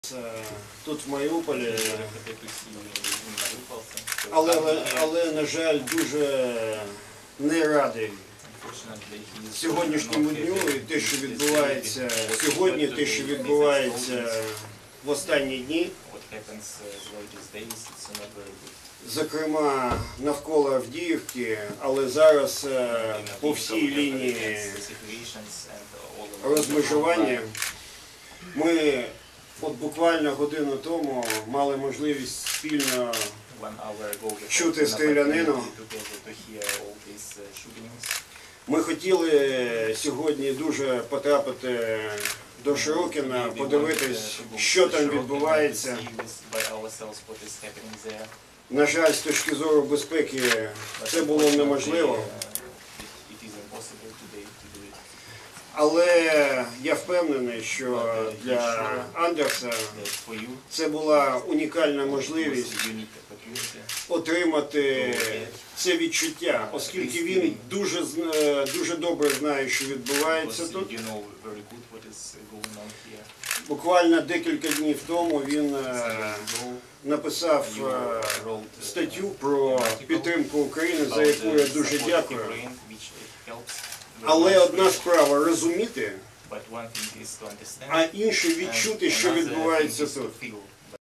Глава МИД Дании посетил Мариуполь
Глава Министерства иностранных дел  Дании Андерс Самуэльсен сказал, что надеется на мирное разрешение ситуации.